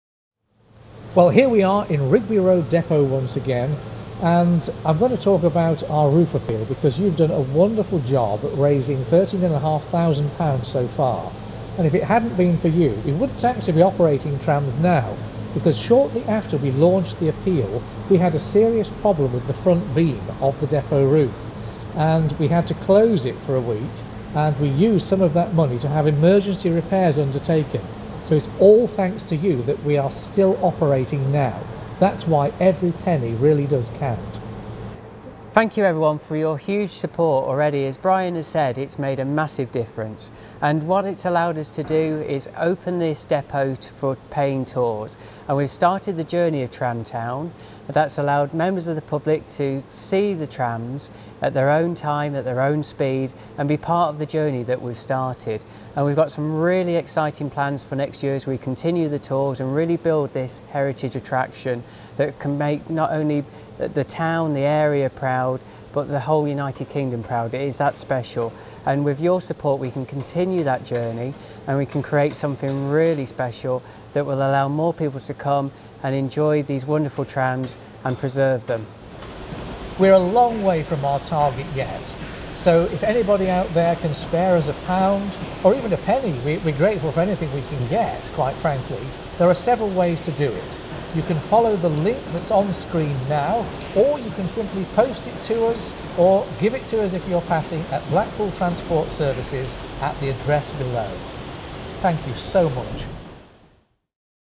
Tramshed Fundraiser Update
An update from Byran Lindop, Head of Heritage and Paul Galley, Cllr for Anchorsholme on our plans to renovate the Tram Shed roof.

If you can, read our crowdfunder and consider donating if you can